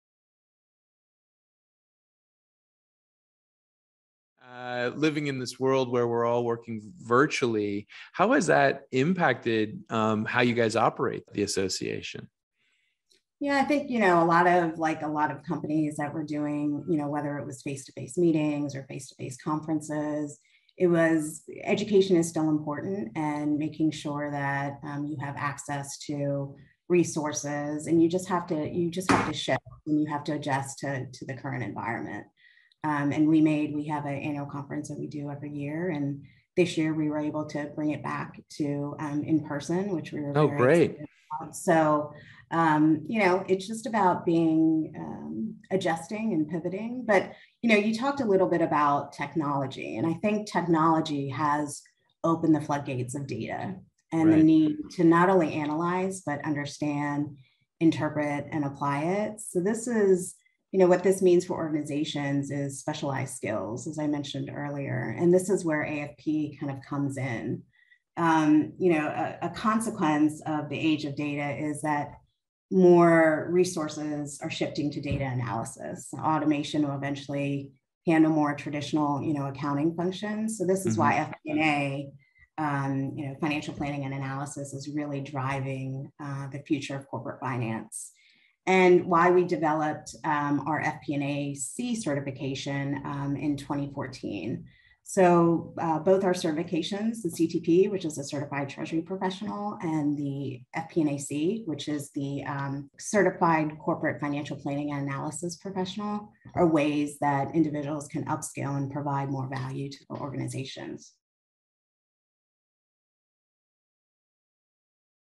Working virtually
Himashi Soriano was on The UNCAGED Show and talked about how is it to work virtually on the Association for Financial Professionals.